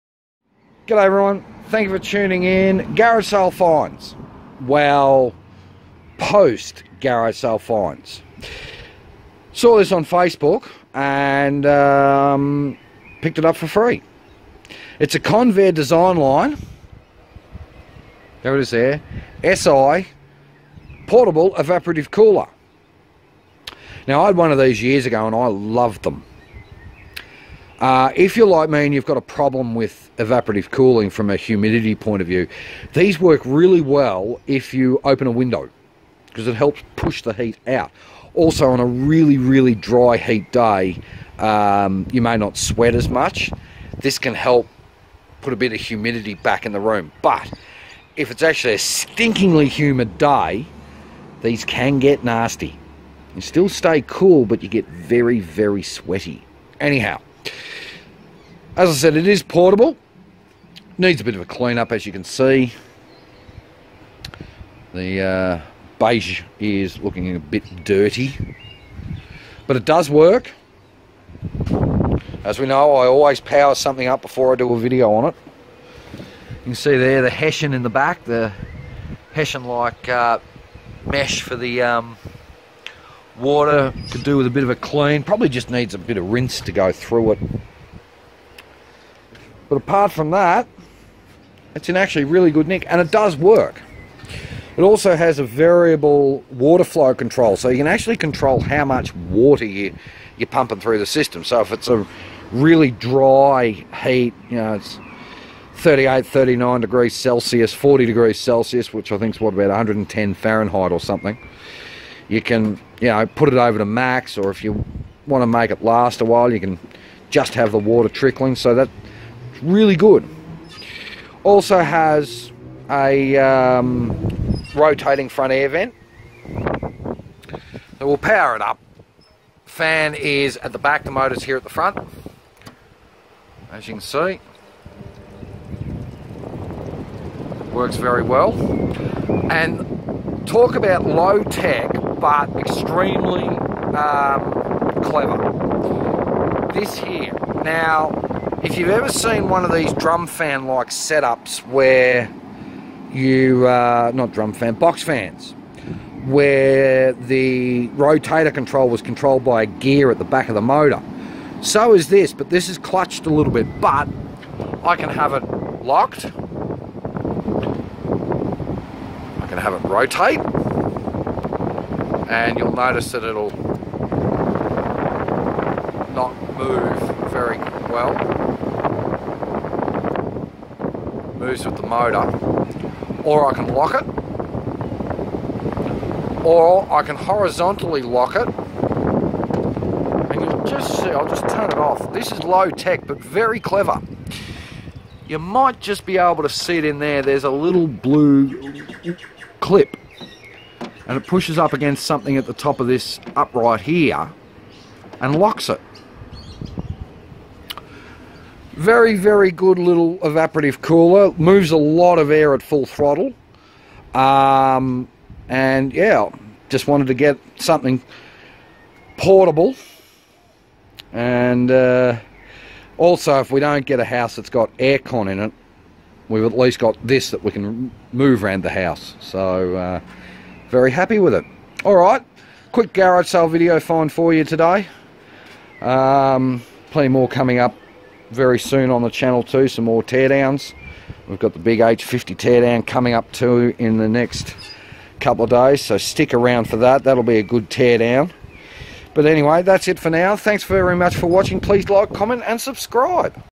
Garage Sale Tech Review - Evap Cooler
Picked this really good evap cooler, so lets have a look